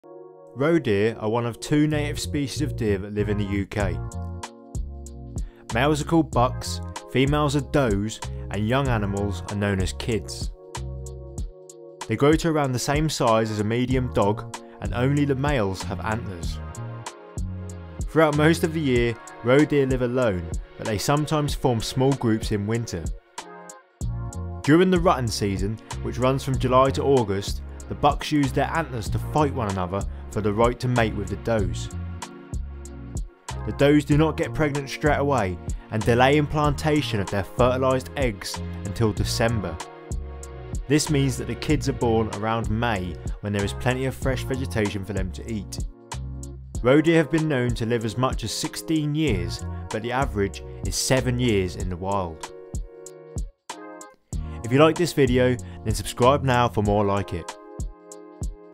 Roe Deer Facts - Deer in the UK Fact file
Roe deer are one of two native species of Deer that live in the UK. Males are called bucks, females are does and young animals are known as kids. They grow to around the size as a medium dog and only the males have antlers. Throughout most of the year roe deer live alone but they sometimes form small groups in winter.
During the rutting season which runs from July to august, the bucks use their antlers to fight one another for the right to mate with the Does. The does do not get pregnant straight away and delay implantation of their fertilized eggs until December. This means that the kids are born around may when there is plenty of fresh vegetation for them to eat. Roe deer have been known to live as much as 16 years but the average is 7 years in the wild.